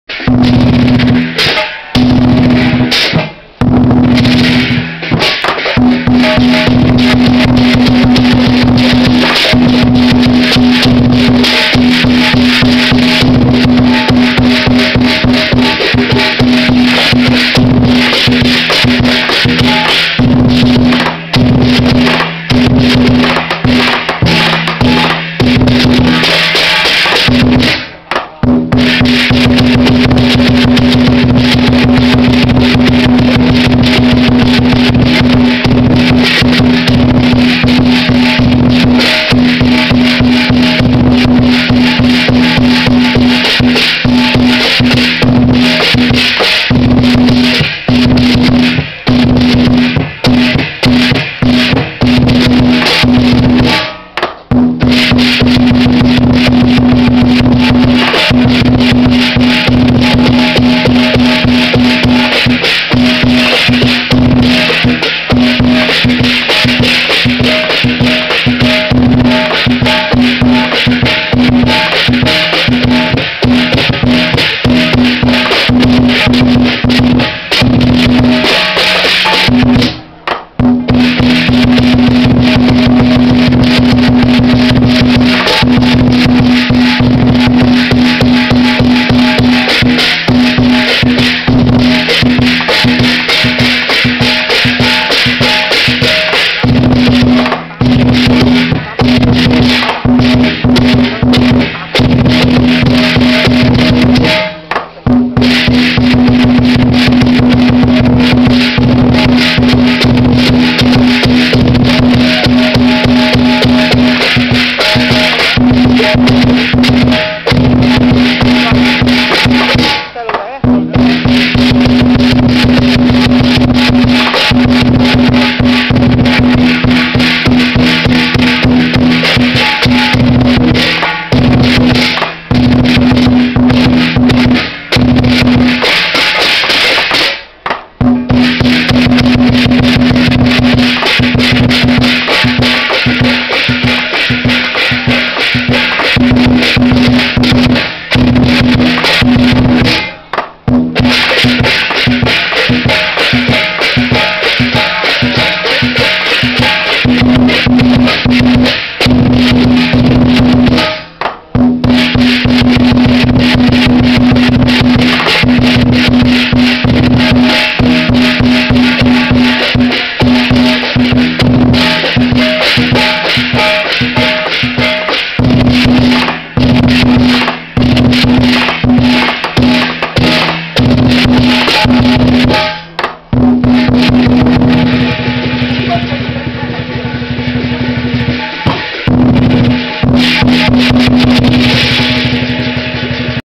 2008 香港南北國術協會就職典禮醒獅表演/到賀 2
2008 香港南北國術協會就職典禮醒獅表演/到賀(2)